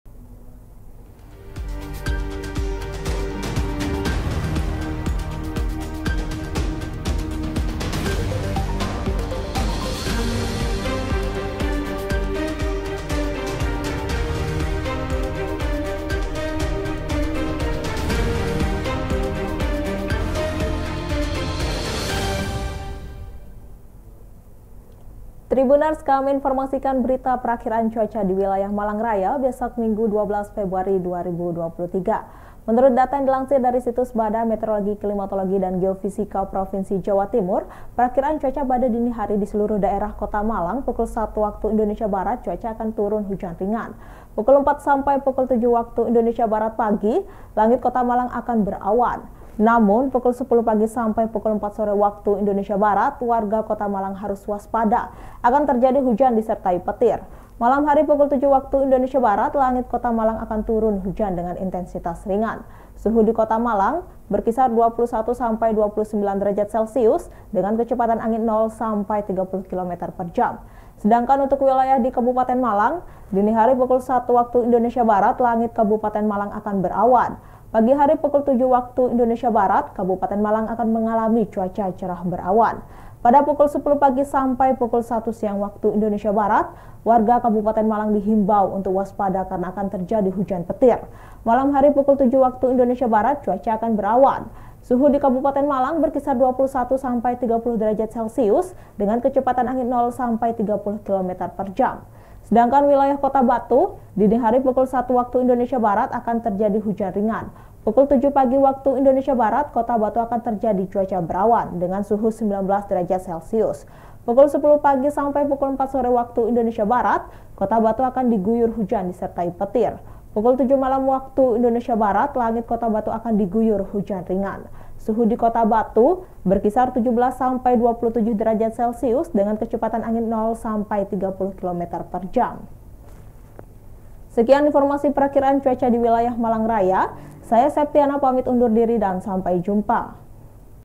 BMKG: WASPADA HUJAN DISERTAI PETIR DI WILAYAH MALANG RAYA, JANGAN MENJAGA KESEHATAN
TRIBUNJATIMTIMUR.COM -